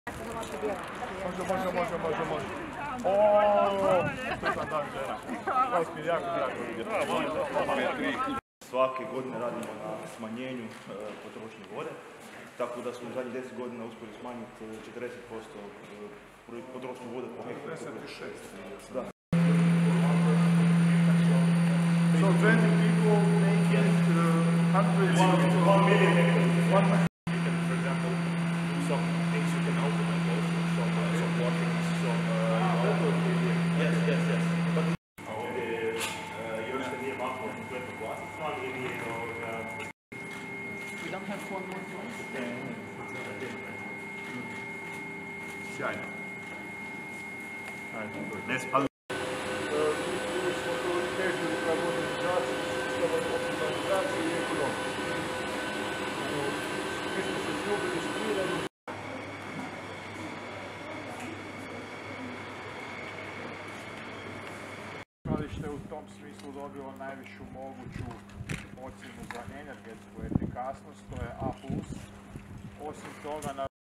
Ministar Ćorić obišao proizvodni pogon tvrtke Heineken Hrvatska
Ministar gospodarstva i održivog razvoja Tomislav Ćorić obišao je proizvodni pogon tvrtke Heineken Hrvatska.